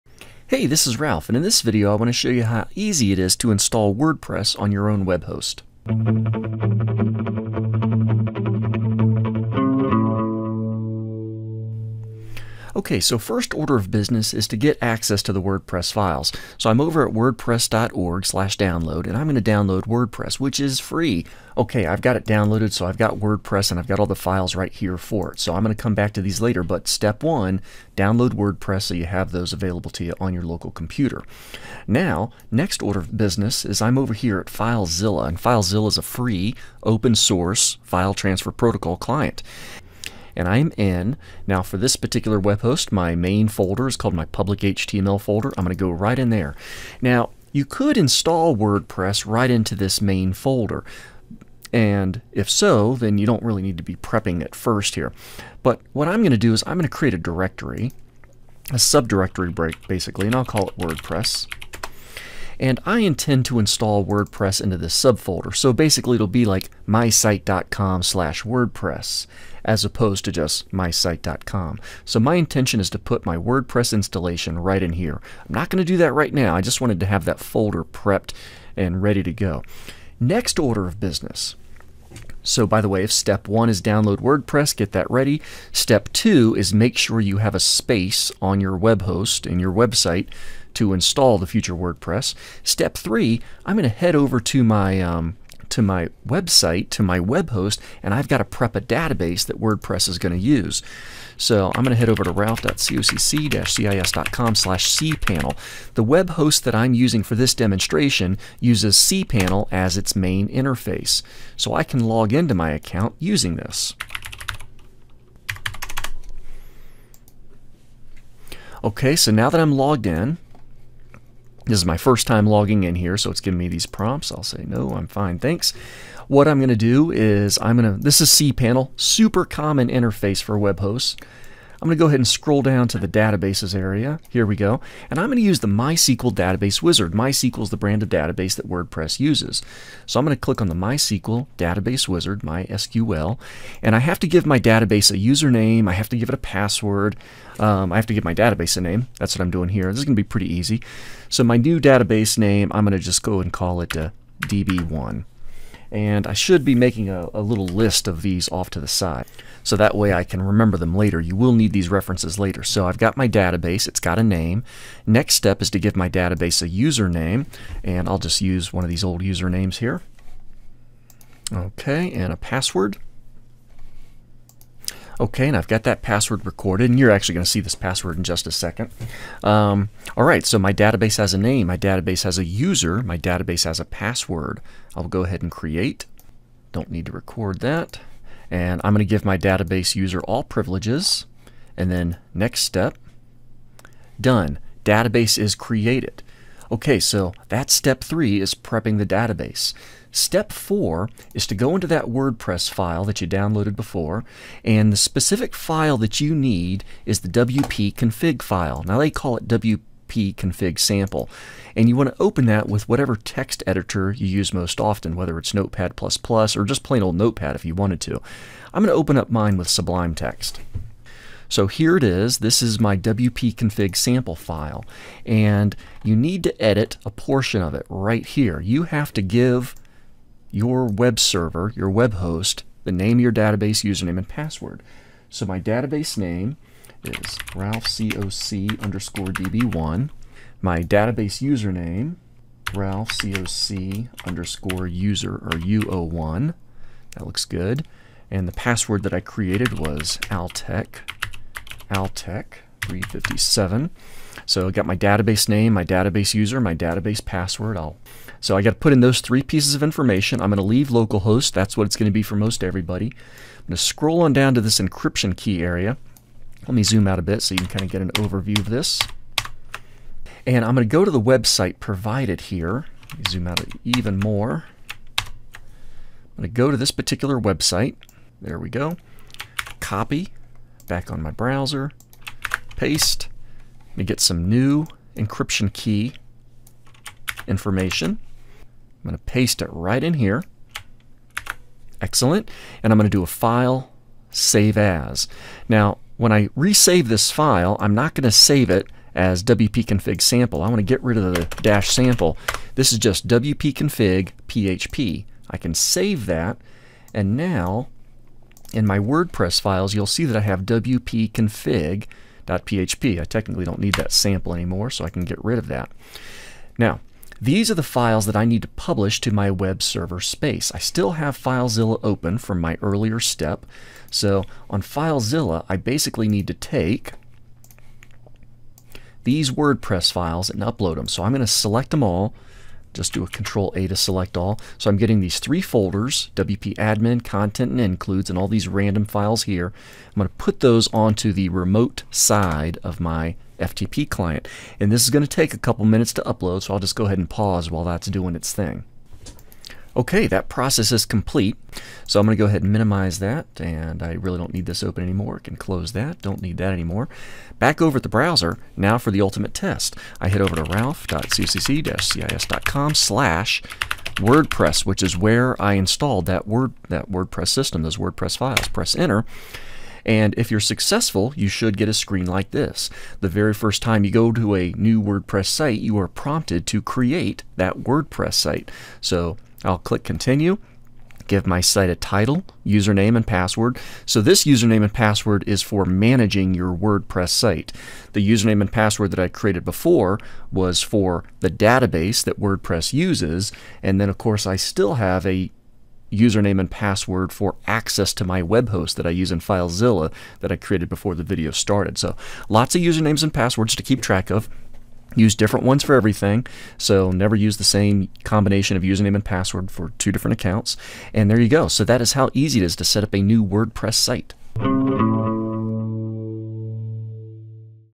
Install and Create a New WordPress Site On Your Web Hosting Service
Review the key steps for creating a new WordPress site if you have web hosting already. The web host used in this demonstration utilizes the cPanel interface, common to many hosting services.